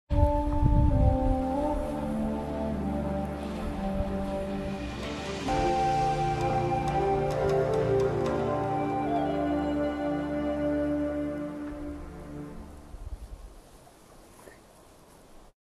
River Otters - Why are they so Charming?
A very short clip of a river otter rubbing in the snow.